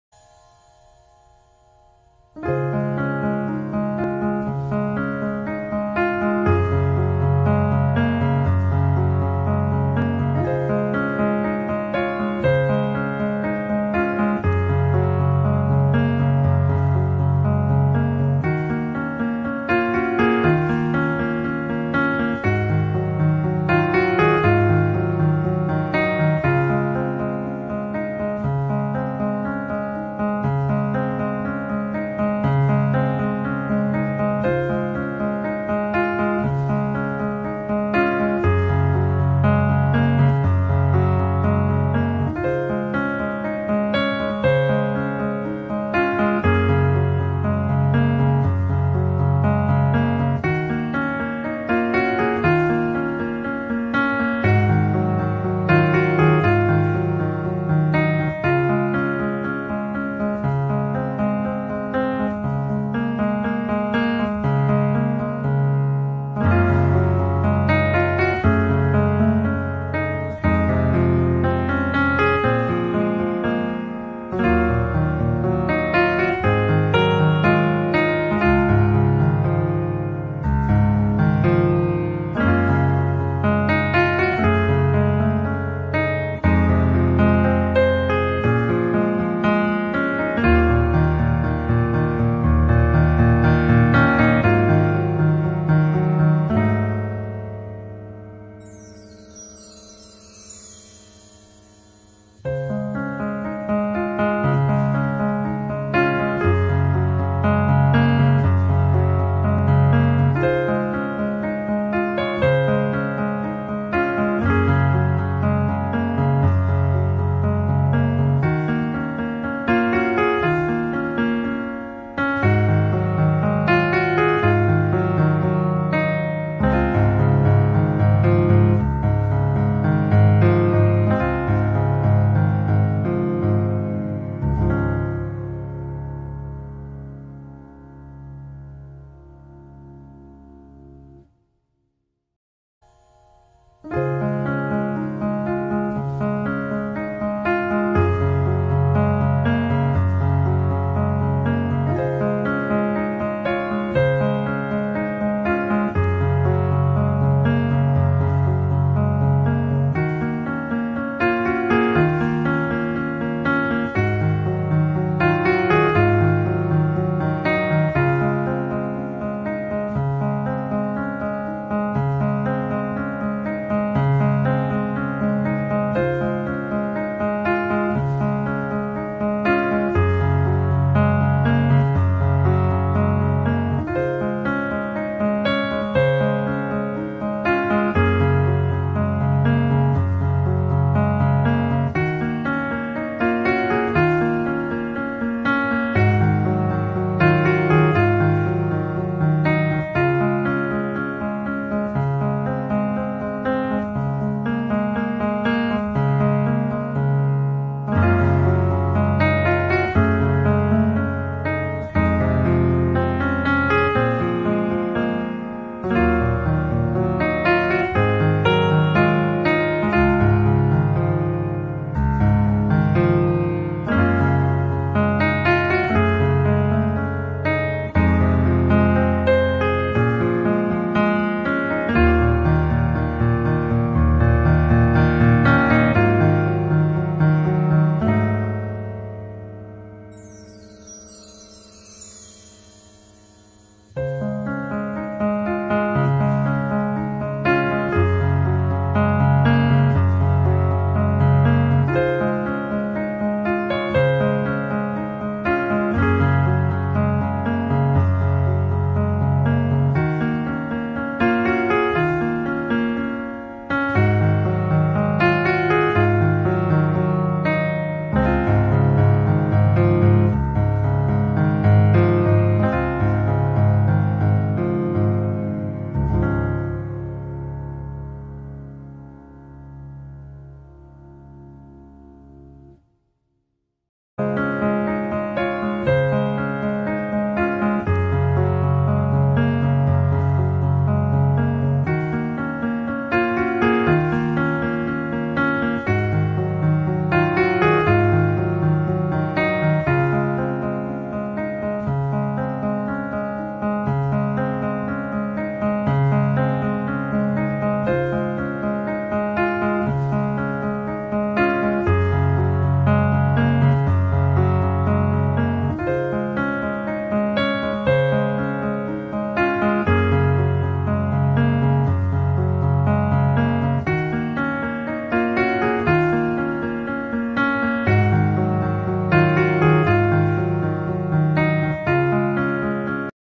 मटर आलू की सब्जी इस तरह से बनाए लोग आपकी तारीफ करेंगे।Matar Aalu Recipe। मटर आलू बनाने की विधि।
Incridiant
हरा मटर(Green pea)
आलू(potato)
प्याज (onion)
टमाटर (Tomato)
जीरा (cumin powder)
धनिया (coriander powder)
गोलकी (Black Paper)
गरम मसाला (garam masala)
कश्मीरी लाल मिर्च (kadhmiri red chilli)